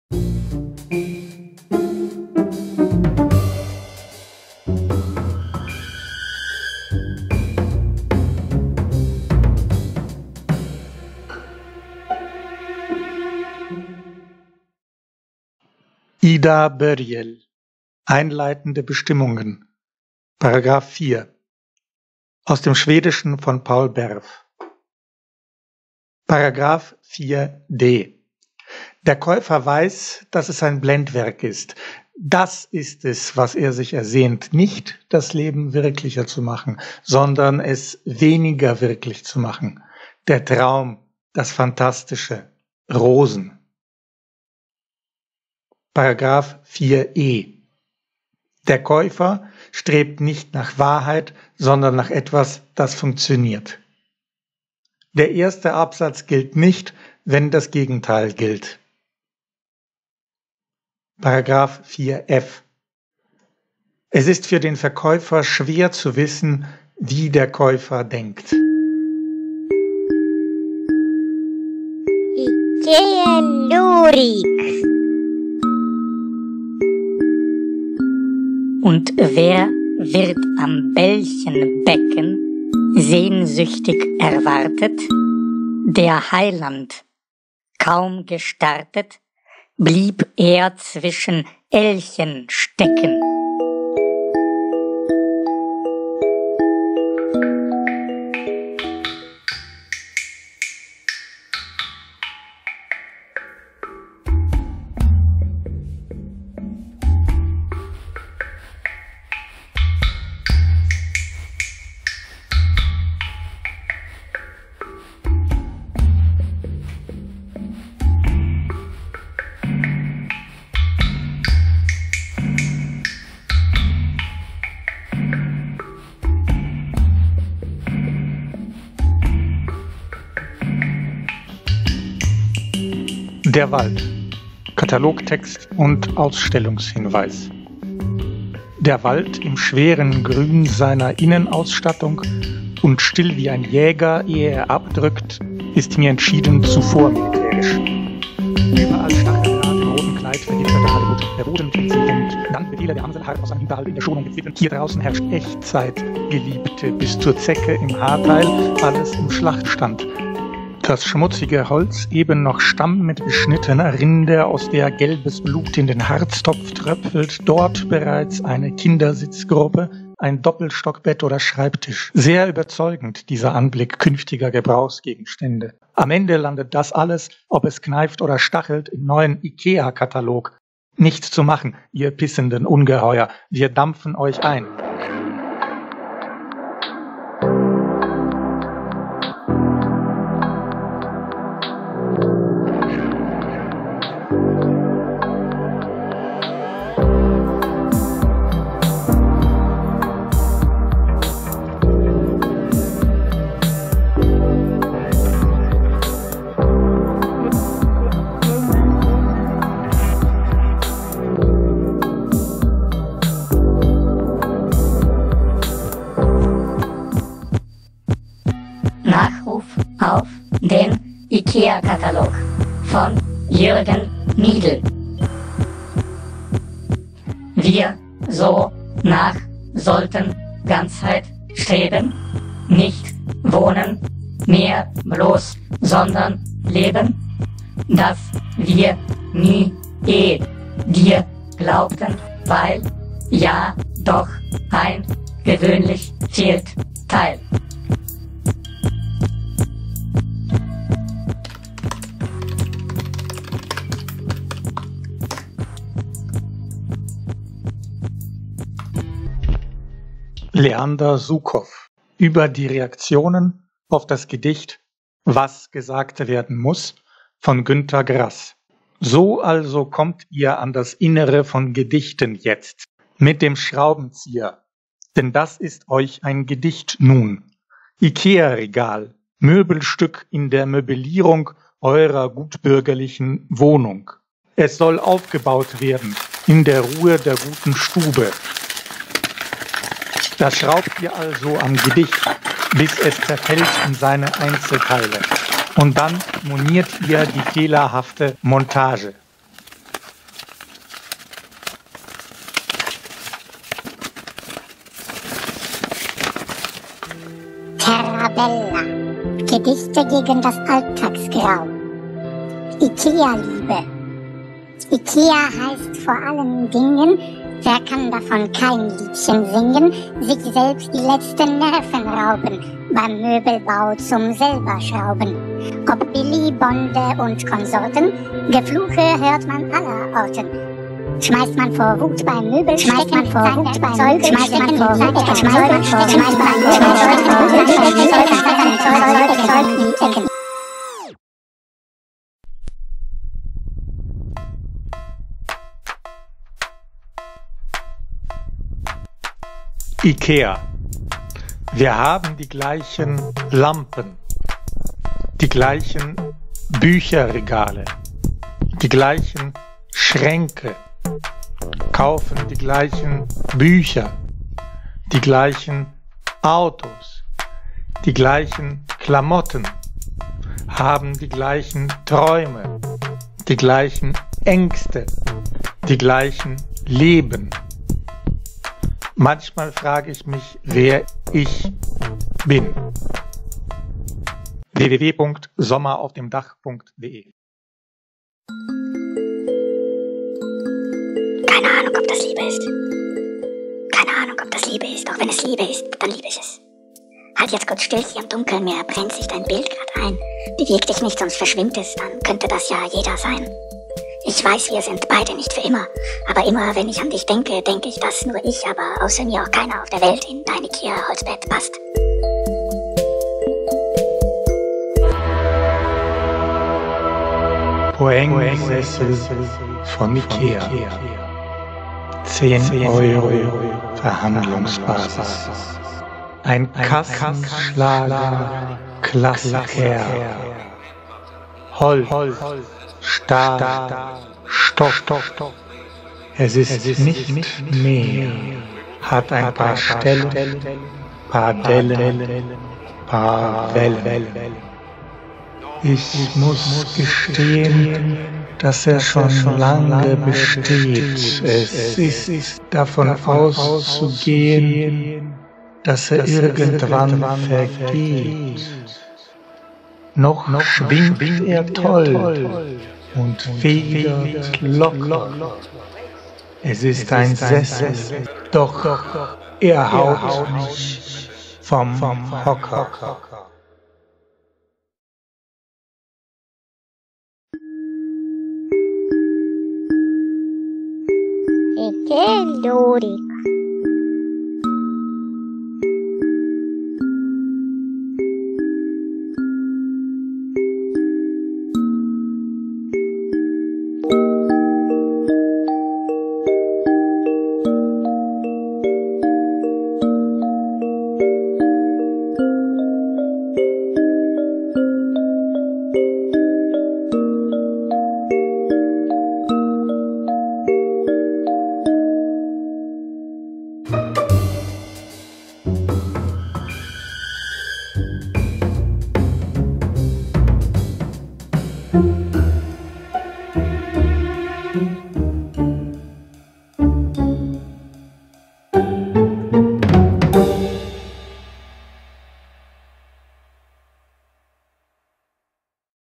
Akademie für Lyrikkritik – Fokus Schweden: Alexander Estis zu Ida Börjel
Lecture Performance zu verschiedenen Gedichten u.a.:
Ida Börjel: "Einleitende Bestimmungen [4§]", Übersetzung aus dem Schwedischen auf lyrikline.de, in: "Konsumentköplagen: juris lyrik", OEI Editör: Stockholm 2008.